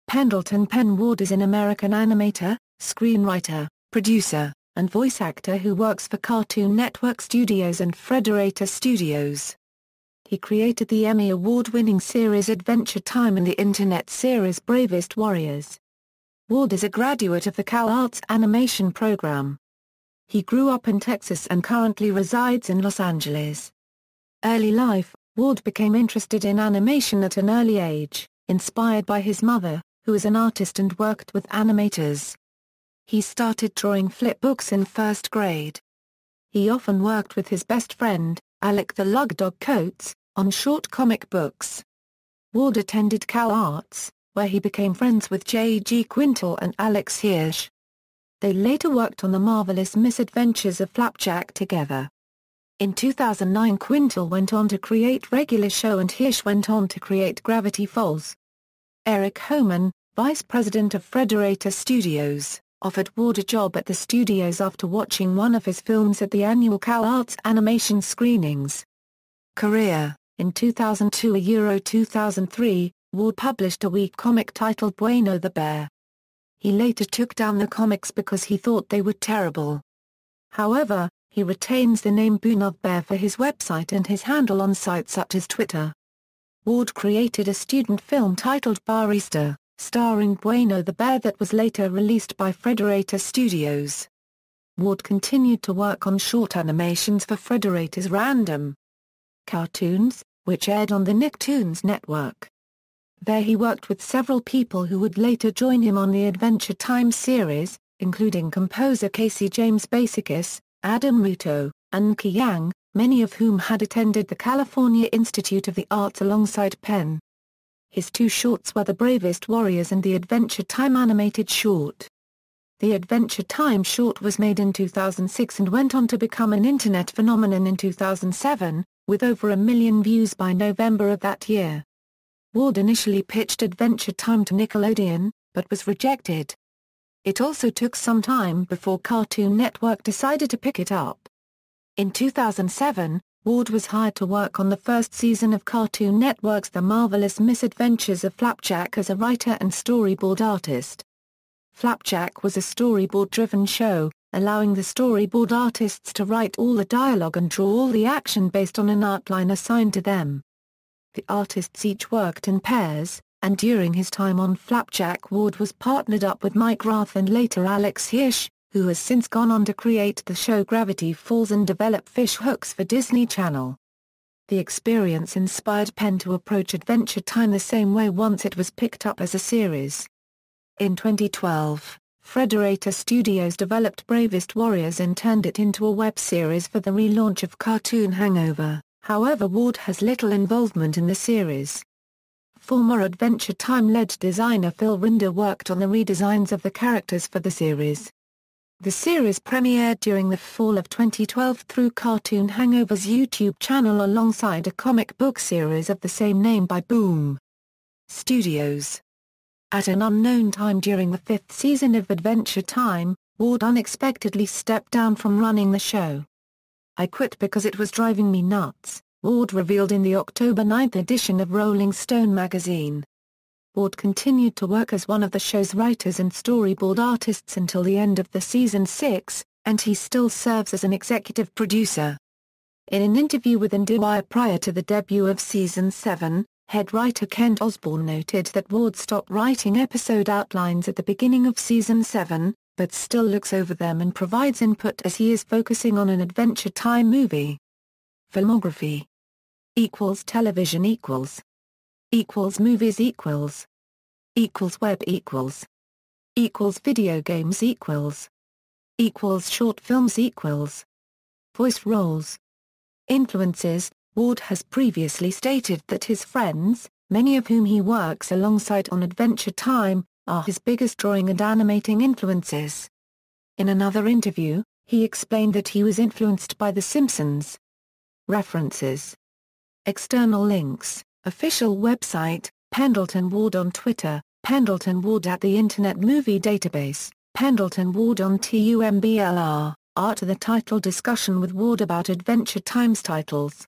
Pendleton Ward
Pendleton "Pen" Ward is an American animator, screenwriter, producer, and voice actor who works for Cartoon Network Studios and Frederator Studios. He created the Emmy-award winning series Adventure Time and the internet series Bravest Warriors. Ward is a graduate of the CalArts Animation Program. He grew up in Texas and currently resides in Los Angeles.